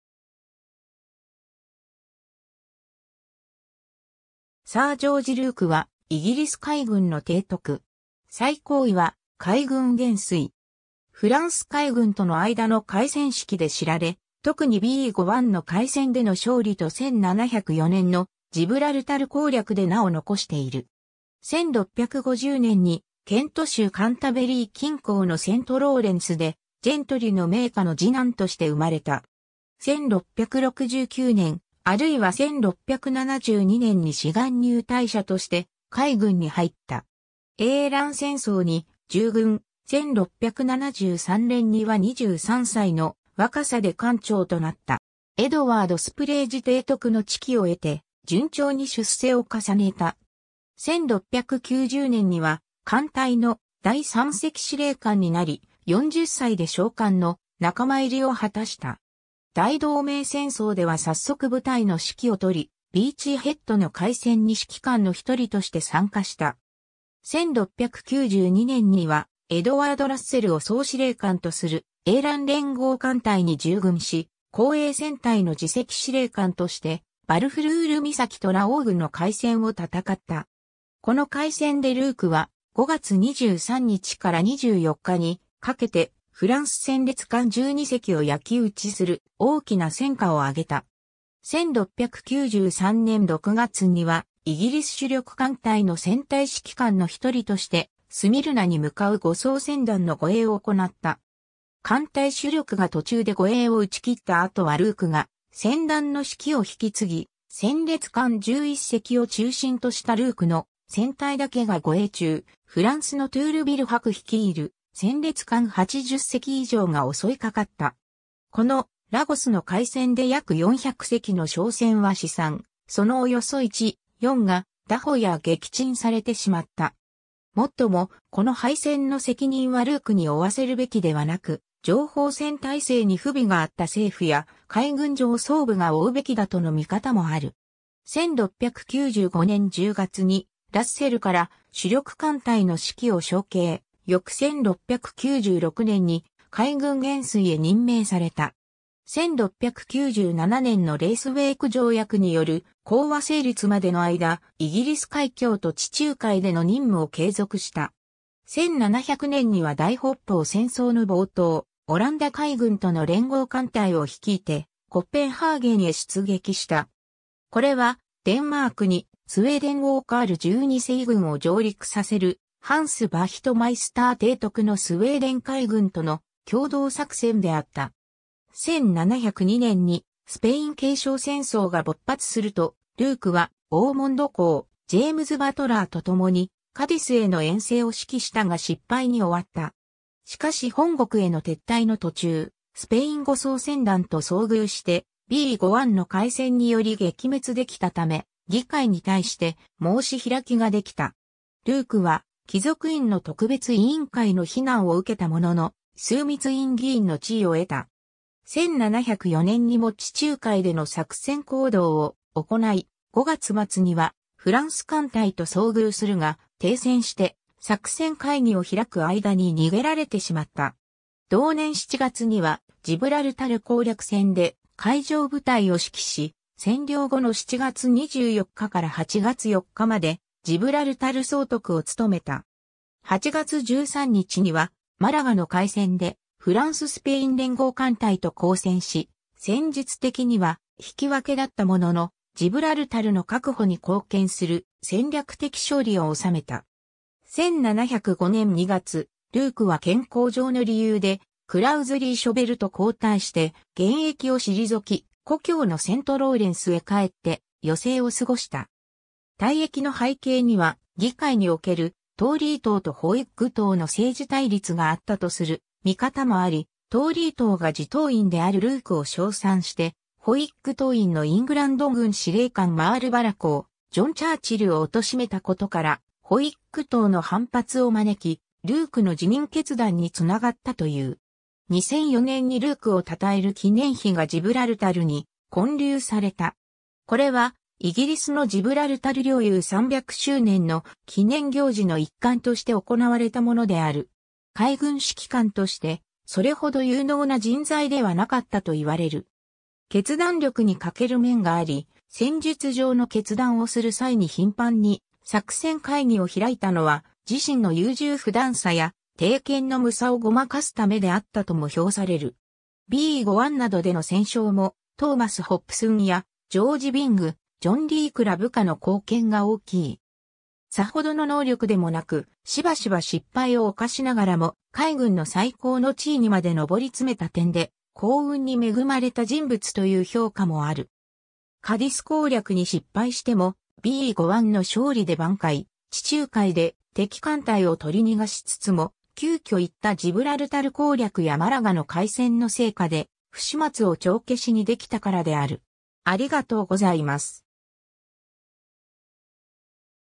ジョージ・ルーク
サー・ジョージ・ルーク（英語: Sir George Rooke, 1650年 - 1709年1月24日）は、イギリス海軍の提督。
最高位は海軍元帥。
フランス海軍との間の海戦指揮で知られ、特にビーゴ湾の海戦での勝利と1704年のジブラルタル攻略で名を残している。
 1650年にケント州カンタベリー近郊のセントローレンスで、ジェントリの名家の次男として生まれた。
1669年、あるいは1672年に志願入隊者（volunteer）として海軍に入った。
英蘭戦争に従軍、1673年には23歳の若さで艦長（post captain）となった。
エドワード・スプレイジ提督の知己を得て、順調に出世を重ねた。
 1690年には艦隊の第三席司令官（Rear Admiral、後の少将相当）になり、40歳で将官の仲間入りを果たした。
大同盟戦争ではさっそく部隊の指揮を執り、ビーチー・ヘッドの海戦に指揮官の一人として参加した。
1692年には、エドワード・ラッセルを総司令官とする英蘭連合艦隊に従軍し、後衛戦隊の次席司令官としてバルフルール岬とラ・オーグの海戦を戦った。
この海戦でルークは、5月23日から24日にかけてフランス戦列艦12隻を焼き討ちする大きな戦果を上げた。
 1693年6月には、イギリス主力艦隊の戦隊指揮官の一人として、スミルナに向かう護送船団の護衛を行った。
艦隊主力が途中で護衛を打ち切った後はルークが船団の指揮を引き継ぎ、戦列艦11隻を中心としたルークの戦隊だけが護衛中、フランスのトゥールヴィル伯率いる戦列艦80隻以上が襲いかかった。
このラゴスの海戦で約400隻の商船は四散、そのおよそ1/4が拿捕や撃沈されてしまった。
もっとも、この敗戦の責任はルークに負わせるべきではなく、情報戦態勢に不備があった政府や海軍上層部が負うべきだとの見方もある。
 1695年10月にラッセルから主力艦隊の指揮を承継、翌1696年に海軍元帥（Admiral of the Fleet）へ任命された。
1697年のレイスウェイク条約による講和成立までの間、イギリス海峡と地中海での任務を継続した。
1700年には大北方戦争の冒頭、オランダ海軍との連合艦隊を率いてコペンハーゲンへ出撃した。
これは、デンマークにスウェーデン王カール12世軍を上陸させる、ハンス・ヴァヒトマイスター提督のスウェーデン海軍との協同作戦であった。
 1702年にスペイン継承戦争が勃発すると、ルークはオーモンド公ジェームズ・バトラーと共にカディスへの遠征を指揮したが失敗に終わった。
しかし本国への撤退の途中、スペイン護送船団と遭遇してビーゴ湾の海戦により撃滅できたため、議会に対して申し開きができた。
ルークは貴族院の特別委員会の非難を受けたものの、枢密院議員の地位を得た。
 1704年にも地中海での作戦行動を行い、5月末にはフランス艦隊と遭遇するが、停船して作戦会議を開く間に逃げられてしまった。
同年7月には、ジブラルタル攻略戦で海上部隊を指揮し、占領後の7月24日から8月4日までジブラルタル総督を務めた。
8月13日にはマラガの海戦でフランス・スペイン連合艦隊と交戦し、戦術的には引き分けだったものの、ジブラルタルの確保に貢献する戦略的勝利を収めた。
 1705年2月、ルークは健康上の理由でクラウズリー・ショヴェルと交代して現役を退き、故郷のセントローレンスへ帰って余生を過ごした。
退役の背景には、議会におけるトーリー党とホイッグ党の政治対立があったとする見方もあり、トーリー党が自党員であるルークを称賛してホイッグ党員のイングランド軍司令官マールバラ公ジョン・チャーチルを貶めたことから、ホイッグ党の反発を招き、ルークの辞任決断につながったという。
 2004年にルークを称える記念碑がジブラルタルに建立された。
これは、イギリスのジブラルタル領有300周年の記念行事の一環として行われたものである。
 海軍指揮官として、それほど有能な人材ではなかったと言われる。
決断力に欠ける面があり、戦術上の決断をする際に頻繁に作戦会議を開いたのは、自身の優柔不断さや定見の無さをごまかすためであったとも評される。
ビーゴ湾などでの戦勝も、トーマス・ホプスン（en）やジョージ・ビング（en）、ジョン・リークら部下の貢献が大きい。
 さほどの能力でもなく、しばしば失敗を犯しながらも海軍の最高の地位にまで上り詰めた点で、幸運...